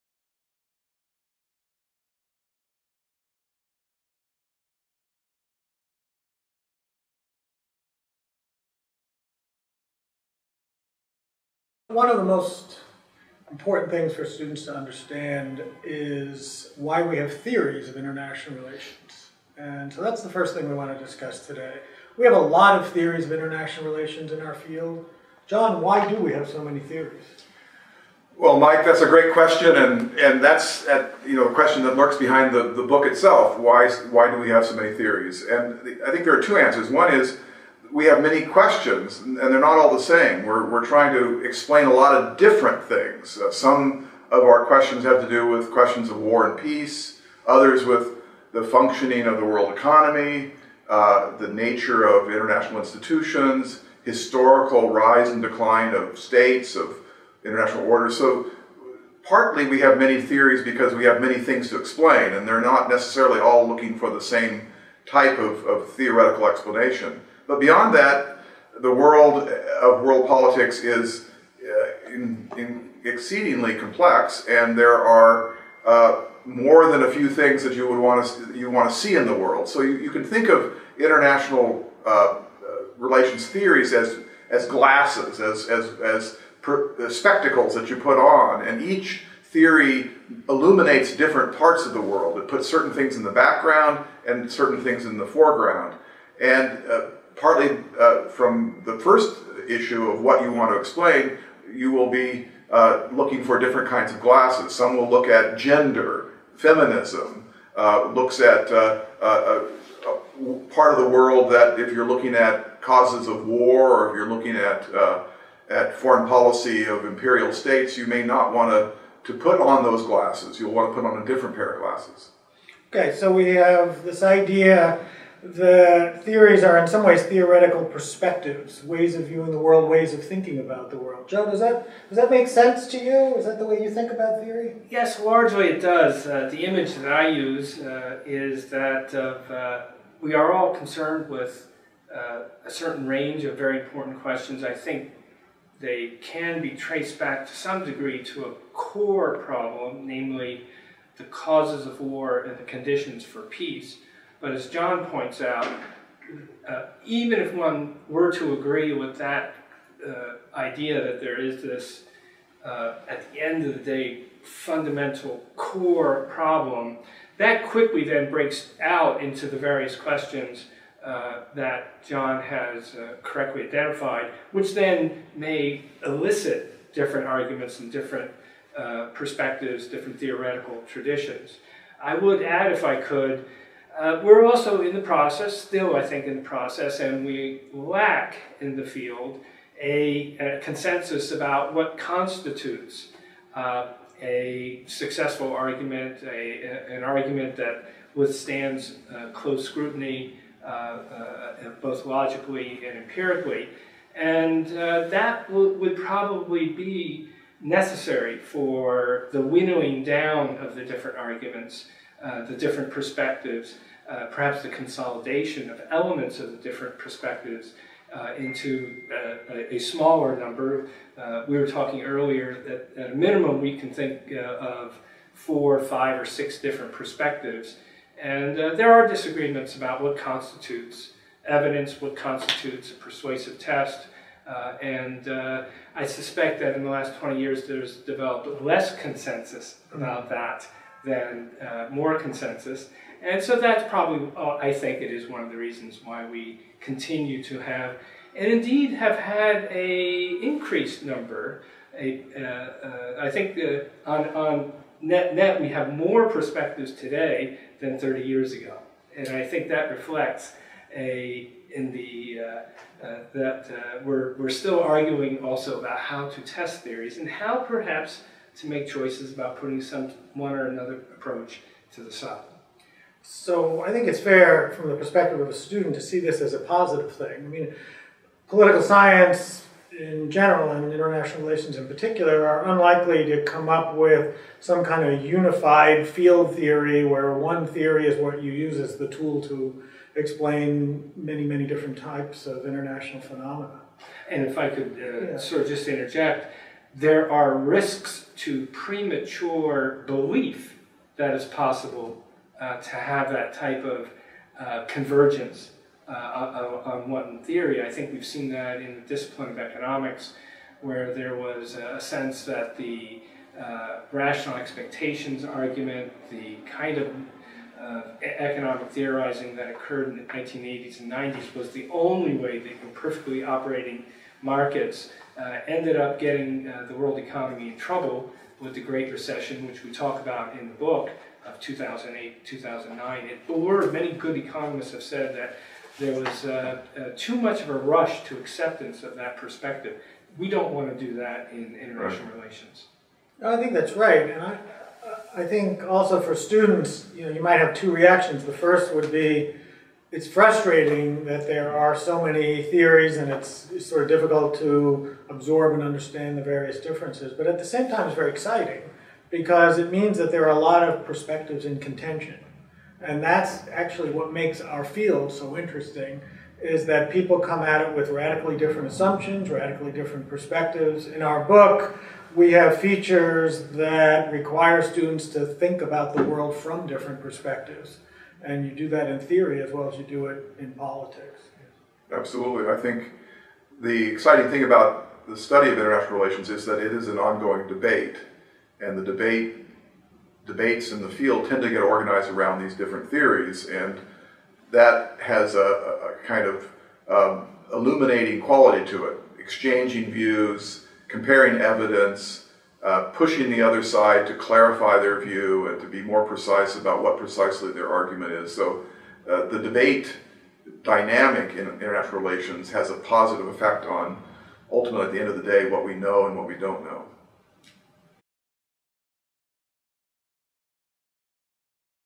Why are there so many theories of International Relations?
Joseph Grieco, G. John Ikenberry, Michael Mastanduno discuss why there are so many theories of International Relations. This conversation accompanies their book "International Relations" (2nd Edition).